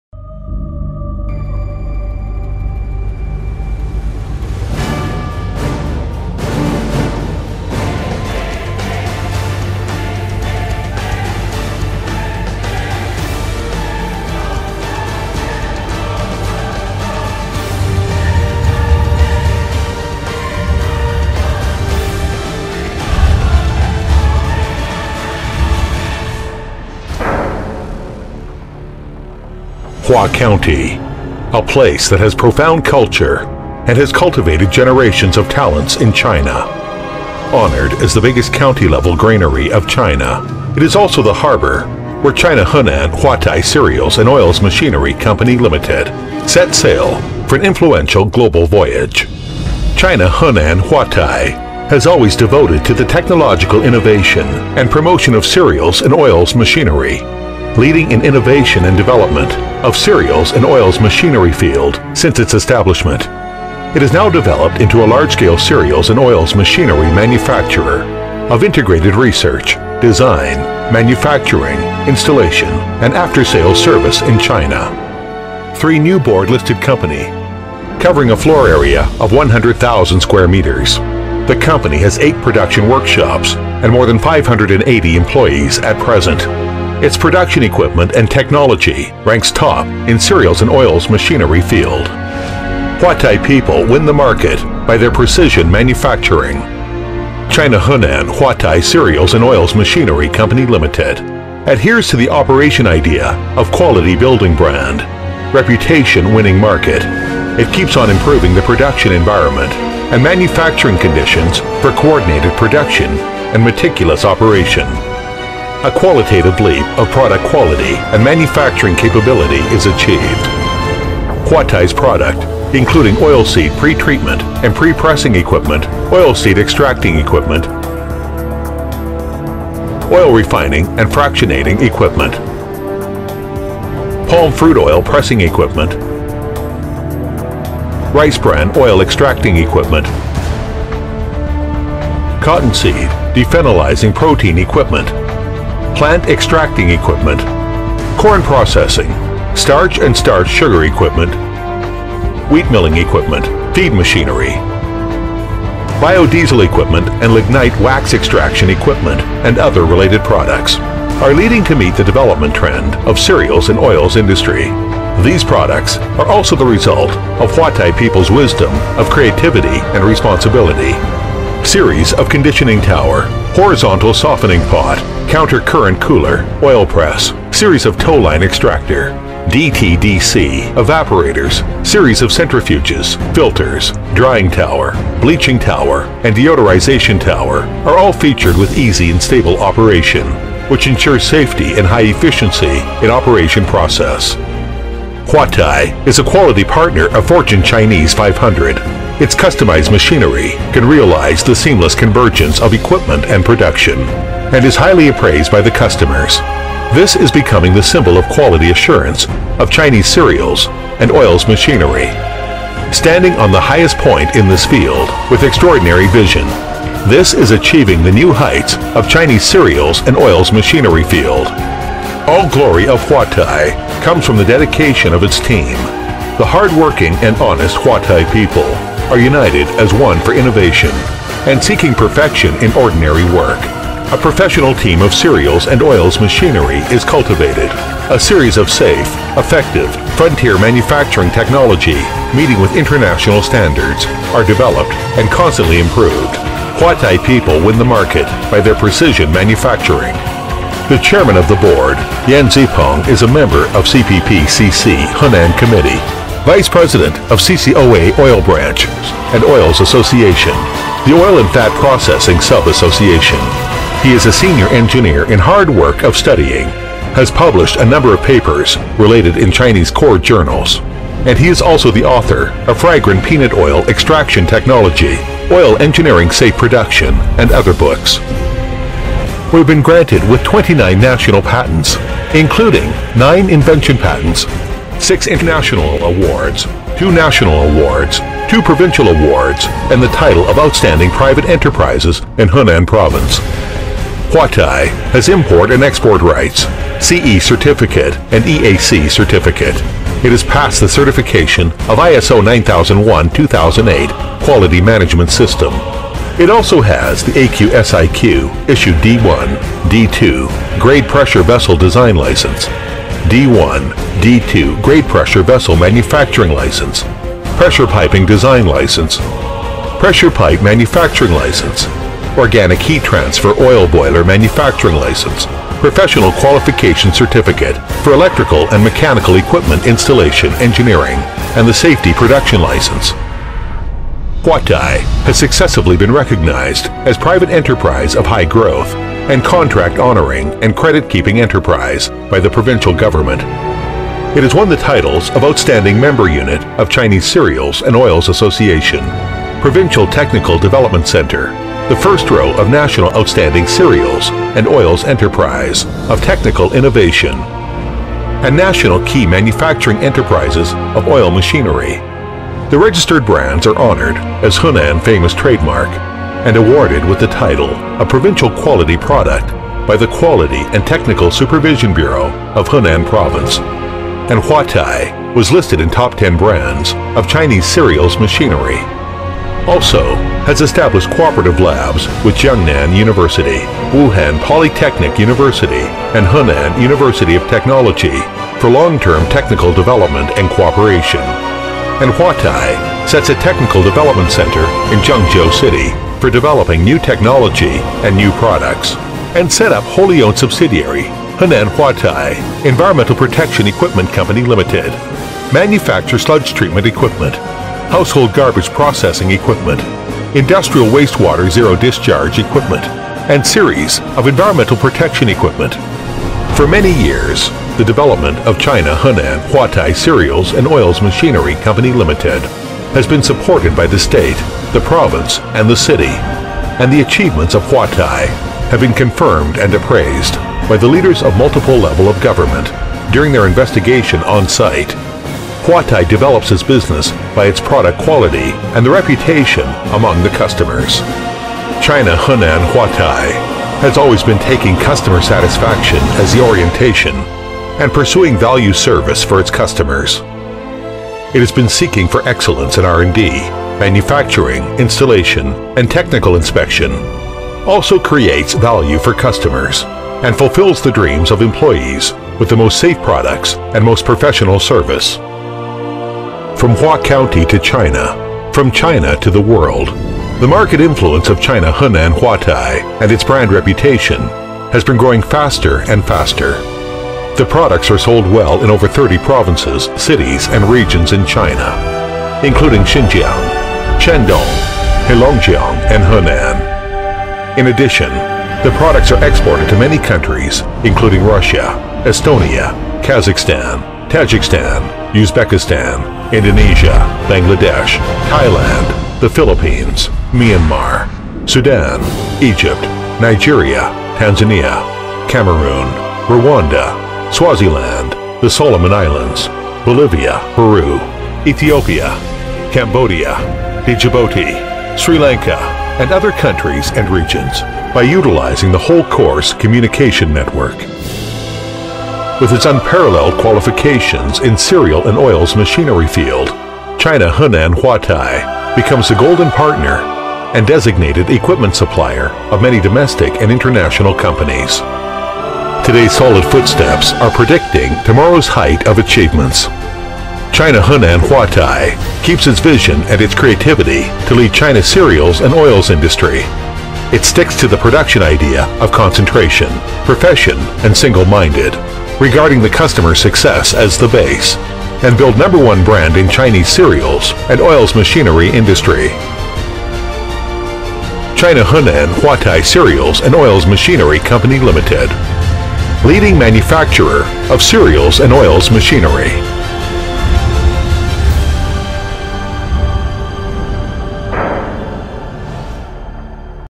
China Henan Huatai Cereals And Oils Machinery Co ,Ltd
Henan Huatai Cereals And Oils Machinery Co.,Ltd. was founded in 1950,whcih is a large-scale oil machinery manufacturing enterprise integrating R & D, design, manufacture and installation as one of the major grain and oil machinery manufacturing enterprises in China.Our company headquartered located in Huaxiang County Industries,Henan Province,China,covering 100,000.00 square meters.